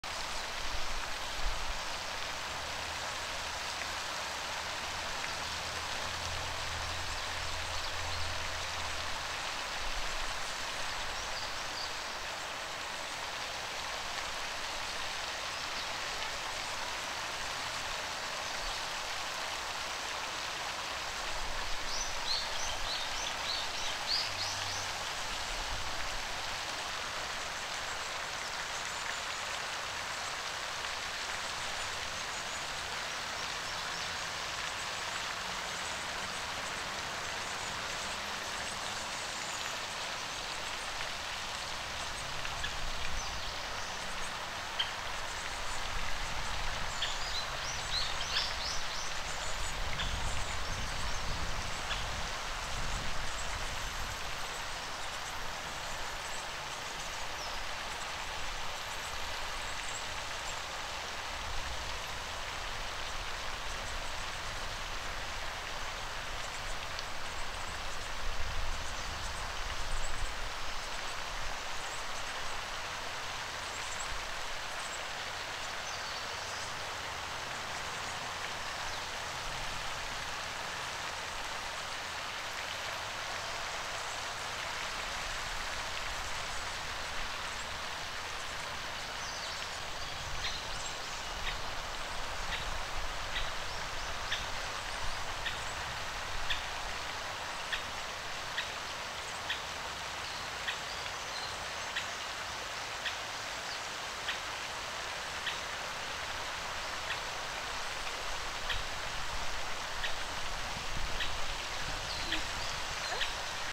Mandarin ducklings resting
While their mon leaves them and go for a meal, they almost stay still and wait for their mom.